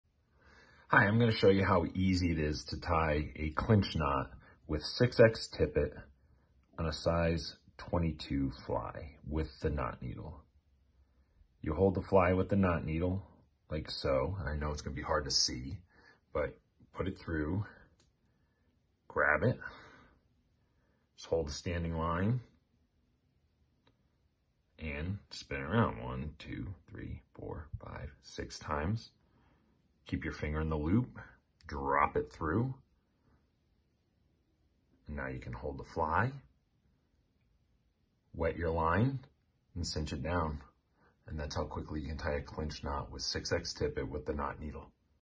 Clinch Knot 6X Tippet 22 Fly using the Knot Kneedle®️
Clinch Knot with 6x tippet and size 22 fly.